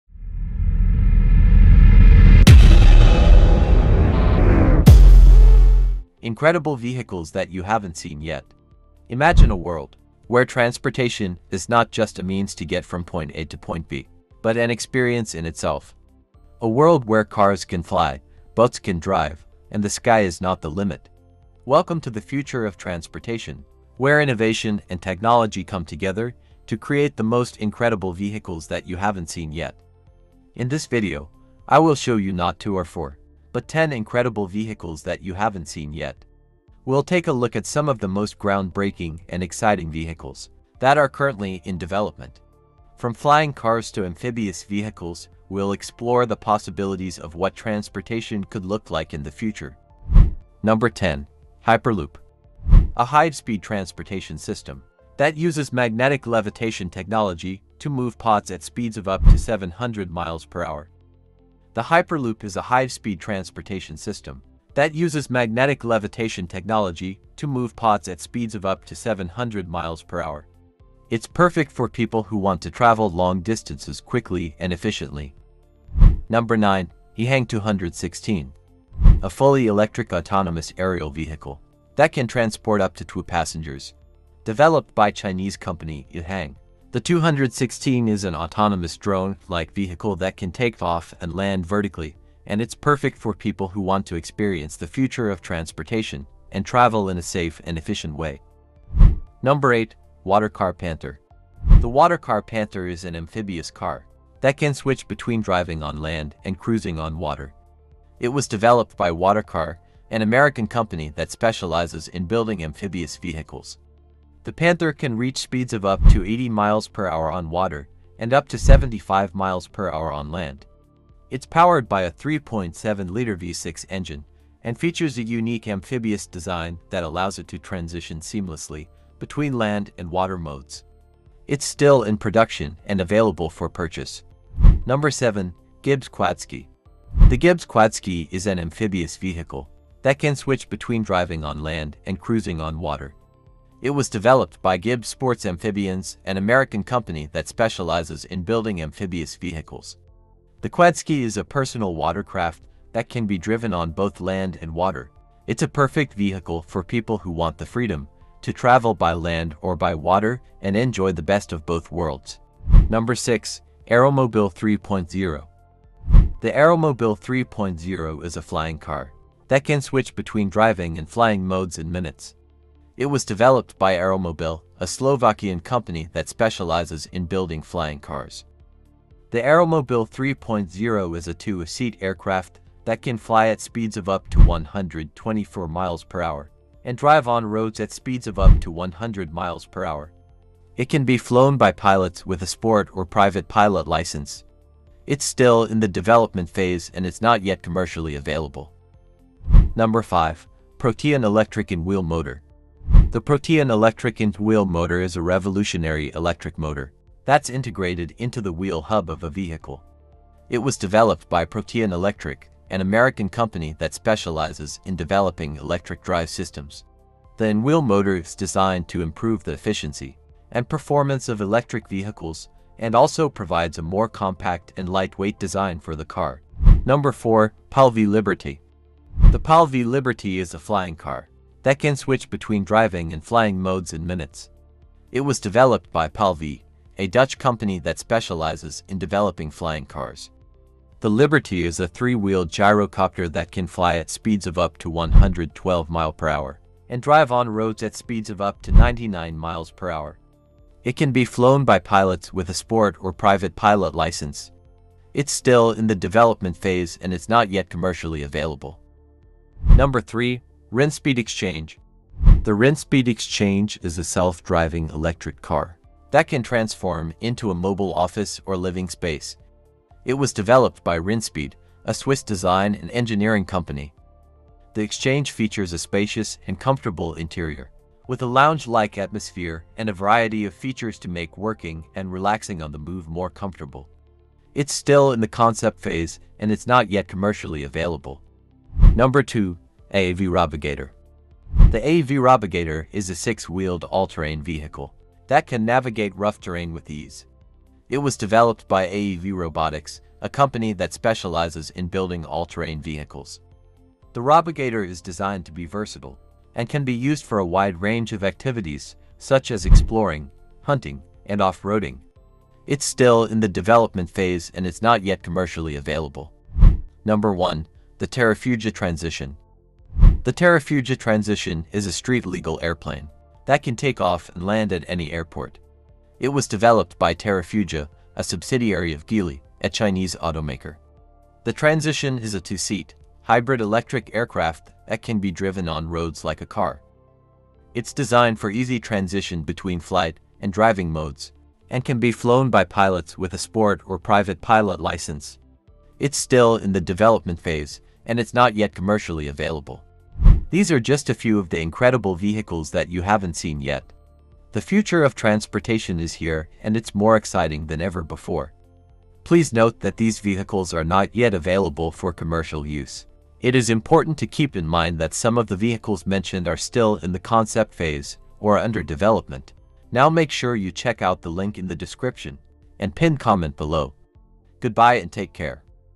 INCREDIBLE VEHICLES THAT YOU HAVEN'T SEEN YET
In this video, we'll take a look at some of the most incredible vehicles that you haven't seen yet. From self-driving cars to flying cars, this video will show you the future of transportation!

If you're interested in the future of transportation, then this video is for you! We'll explore some of the most incredible vehicles that are currently in development, and show you how these vehicles will change the way we move in the future!